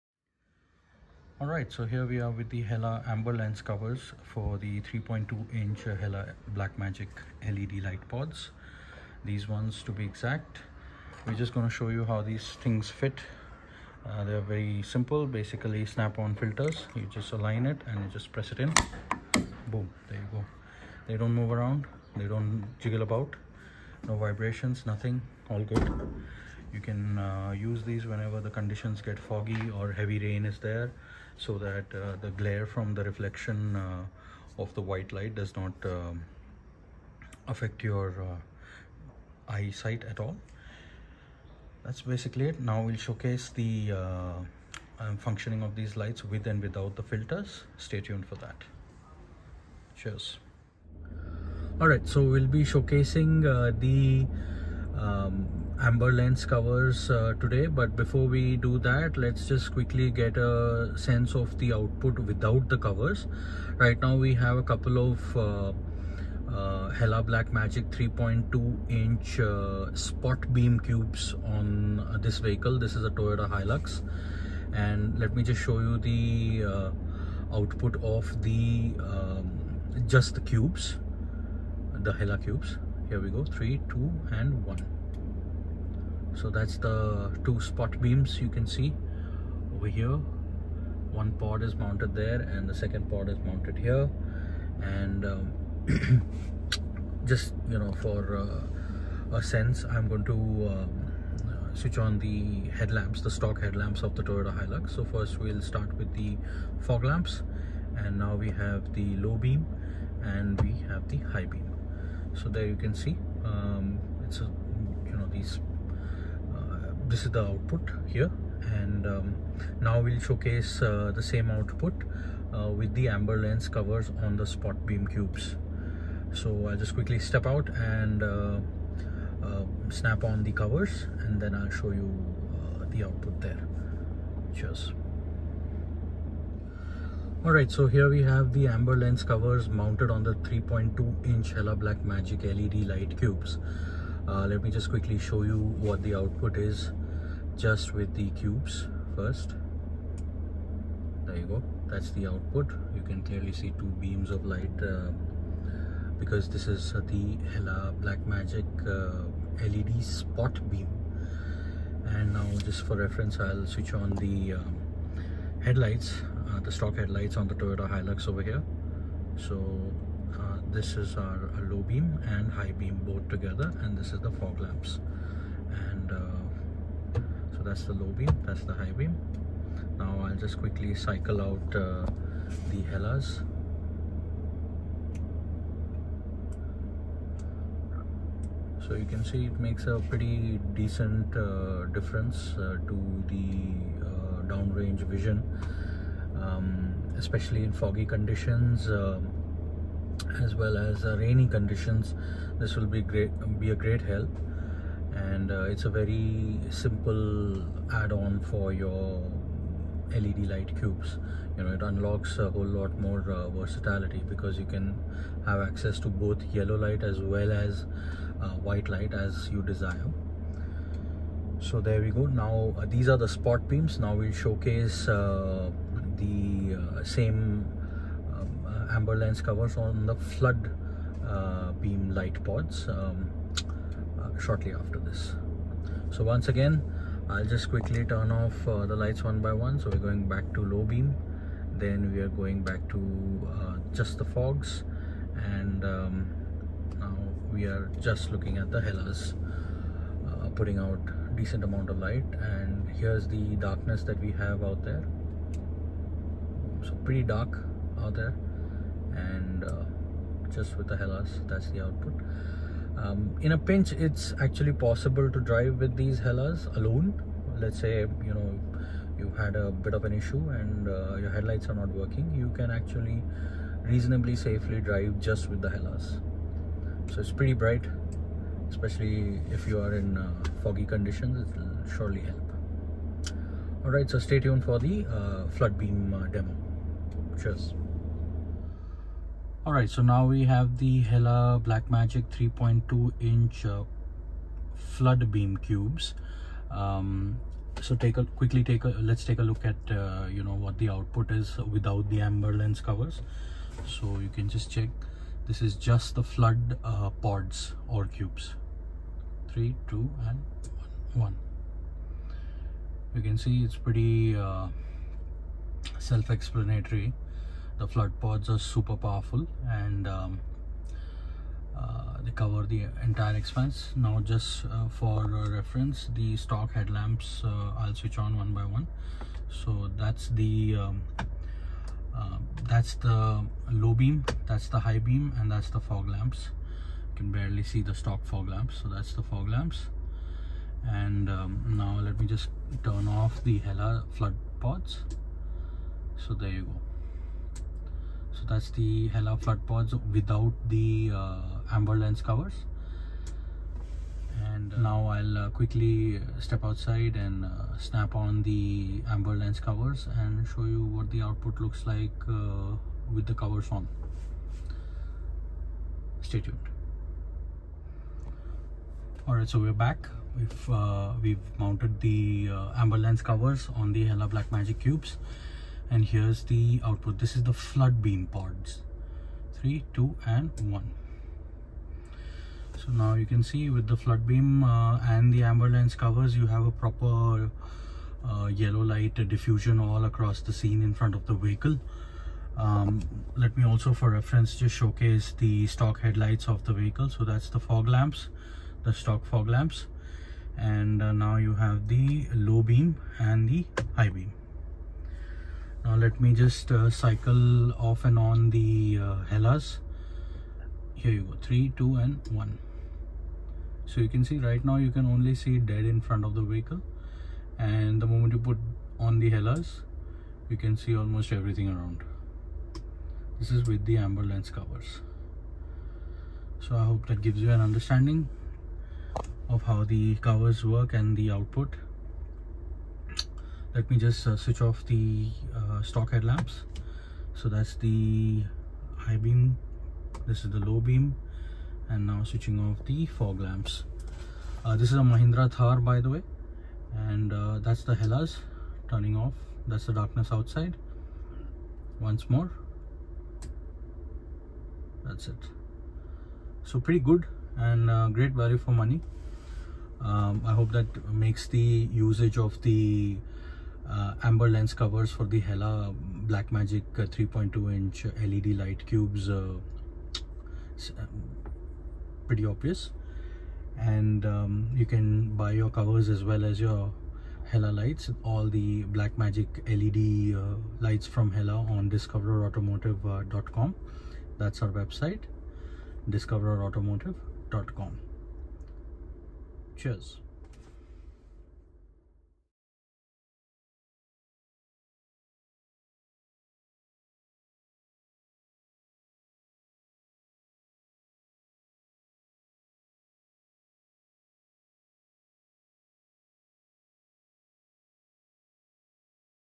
Quick & Dirty Demo | Amber Lens Covers for Hella Black Magic 3 2" LED CUBES | Discoverer Automotive
Hella Amber Lens Covers for BLACK MAGIC LED 3.2" Cubes (Flood & Spot Beams).

Just snap these nifty covers on your Hella LED Cubes and be ready for foggy driving conditions in seconds. Unlock even more value from your already purchased Hella Black Magic 3.2" LED Cubes by adding the Amber Lens Covers.

Sold as a pair. Lights not included.
FITS HELLA BLACK MAGIC 3.2" LED CUBES.

we're all discoverers